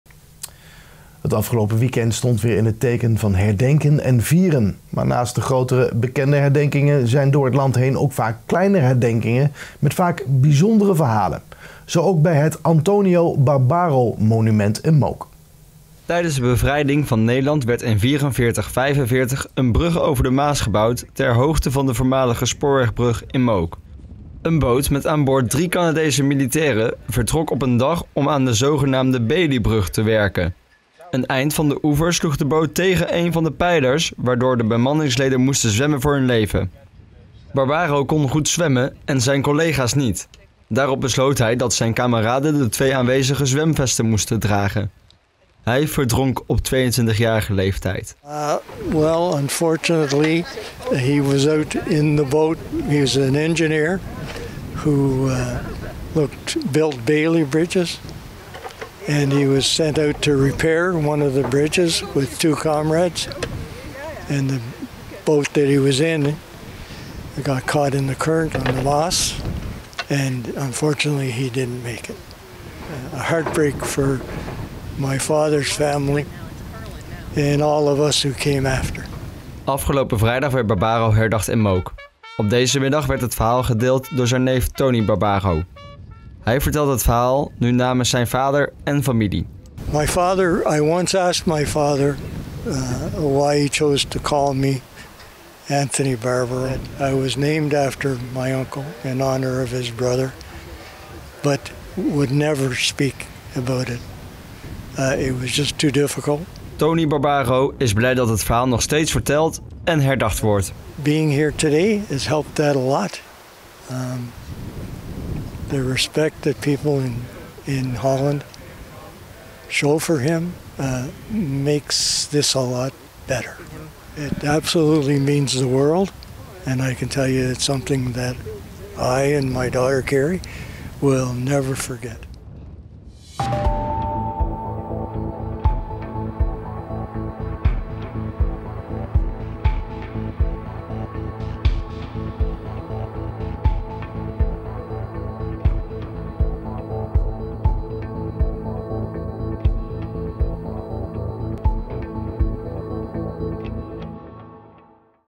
Antonio Barbaro herdacht in Mook || RN7 REGIONIEUWS
Het afgelopen weekend stond weer in het teken van herdenken en vieren. Maar naast de grote bekende herdenkingen, zijn er door het land heen ook veel kleinere herdenkingen met vaak bijzondere verhalen. Zo ook bij het Antonio Barbaro-monument in Mook.